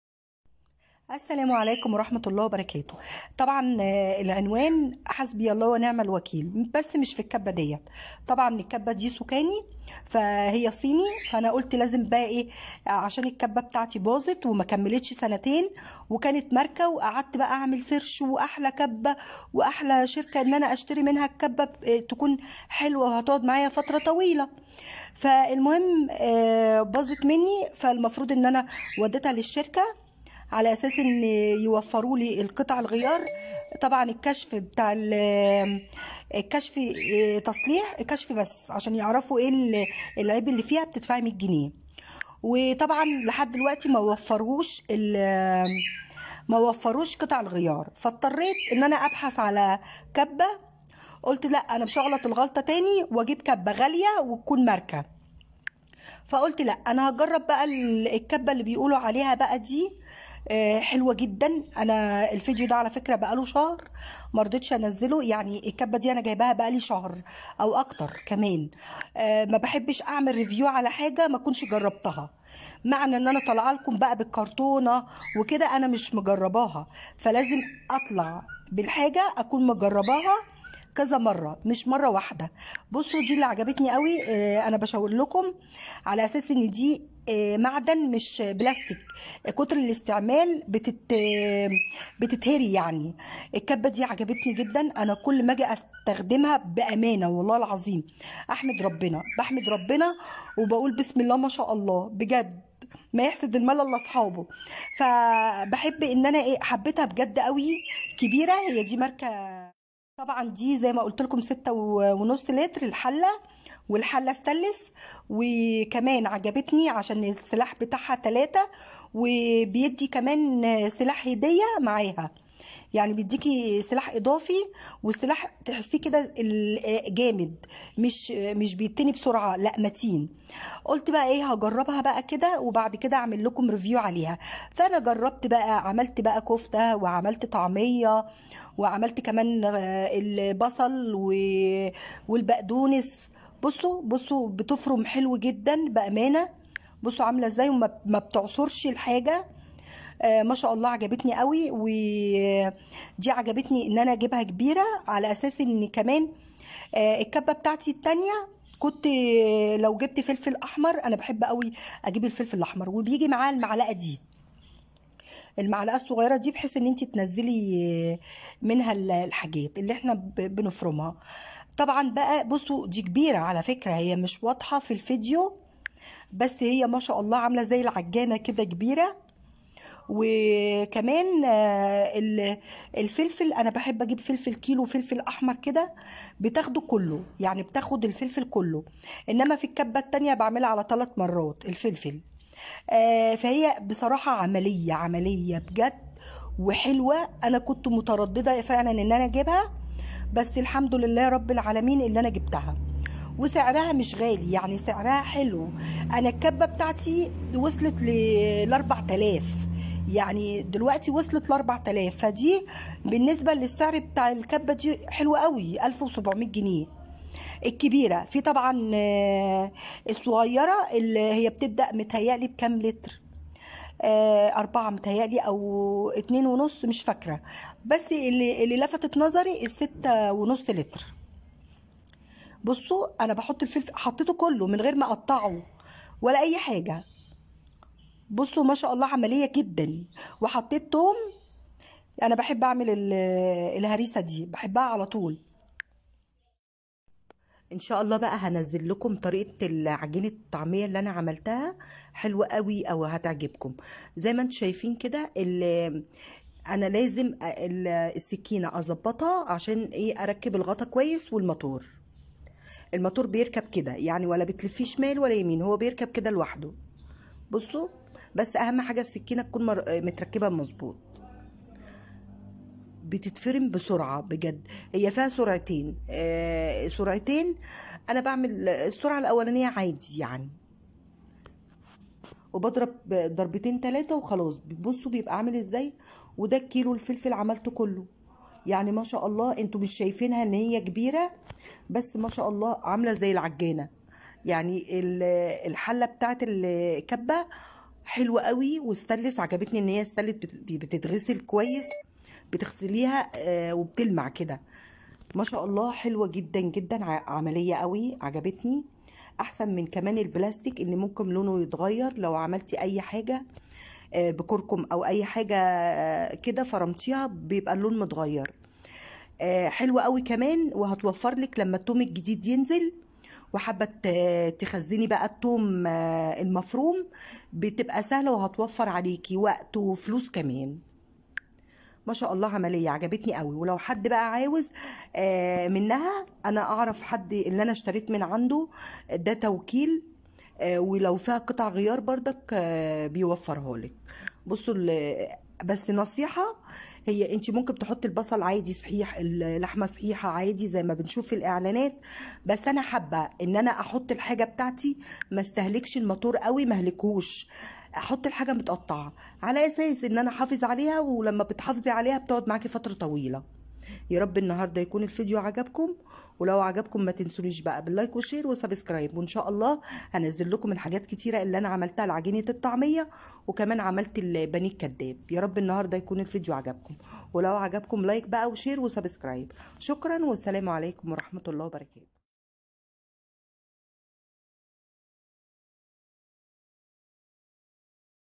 تجربتي مع كبه سوكاني
أول ما لفت انتباهي إلى كبة سوكاني هو أداؤها القوي. فهي مزودة بمحرك بقوة 1500 وات، مما يسمح لها بفرم اللحوم والخضروات بسهولة وسرعة.